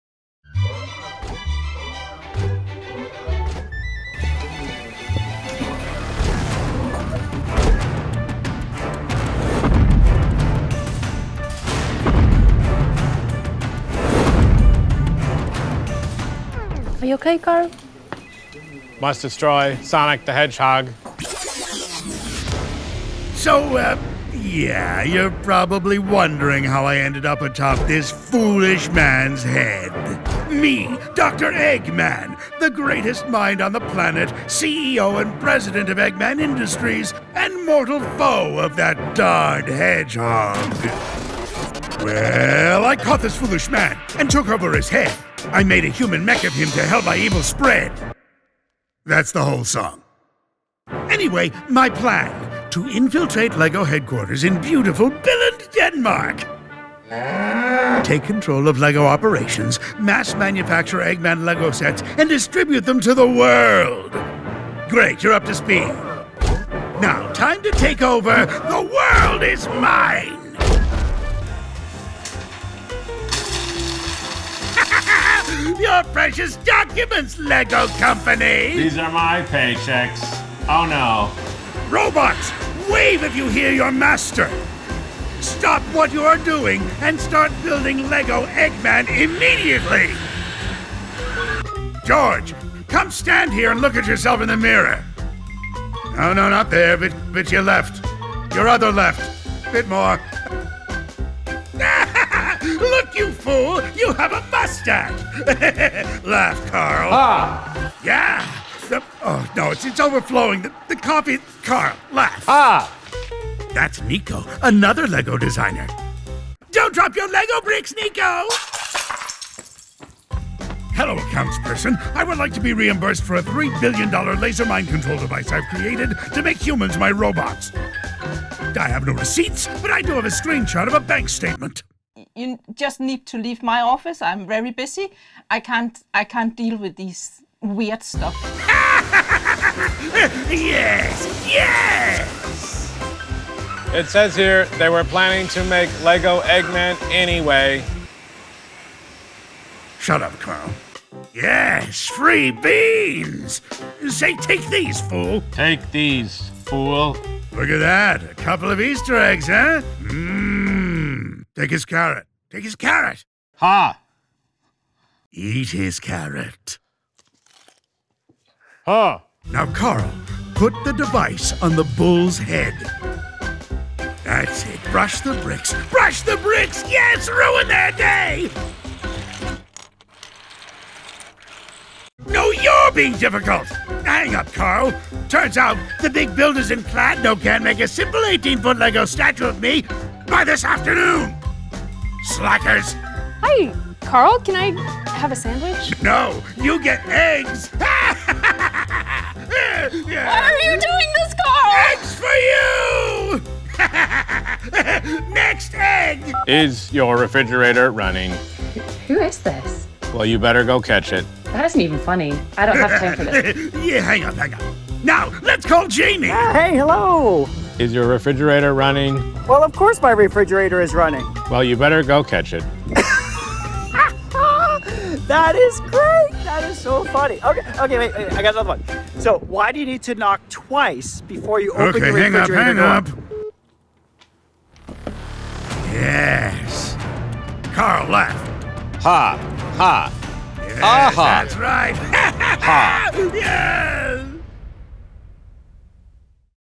Addressing LEGO HQ's New Leadership
Brace yourselves as I, the genius Dr. Eggman, have infiltrated LEGO HQ! I took over the foolish LEGO designer, Carl, to wreak havoc and spread my intellect across this office. A brilliant success!